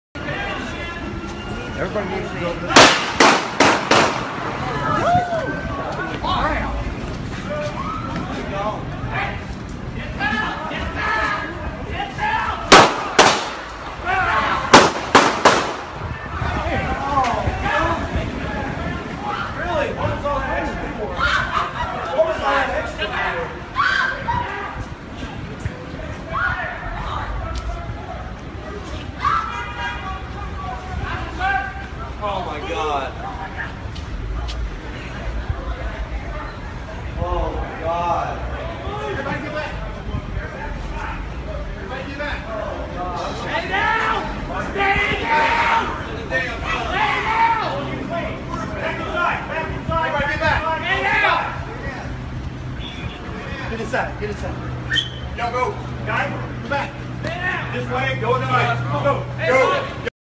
LOVE FIELD SHOOTING
Footage of the shooting at Love Field Airport in Dallas. Captured by Bryan Armstrong and posted to Instagram @flashyfilms